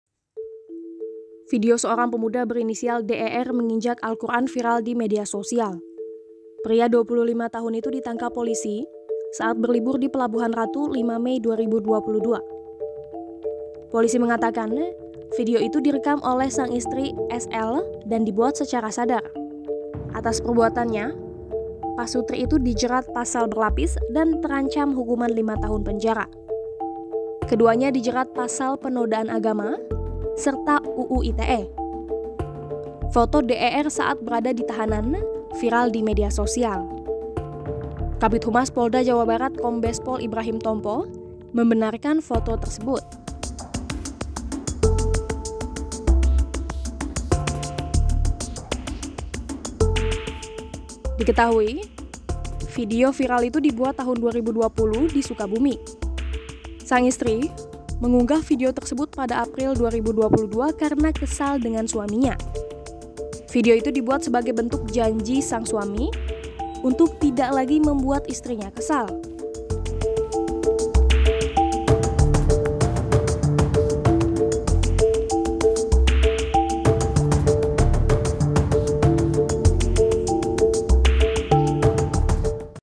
Kepala Plontos, Begini Kondisi Terbaru Pria Injak Alquran Dalam Tahanan
Atas perbuatannya, pasutri dijerat pasal berlapis dan terancam hukuman 5 tahun penjara.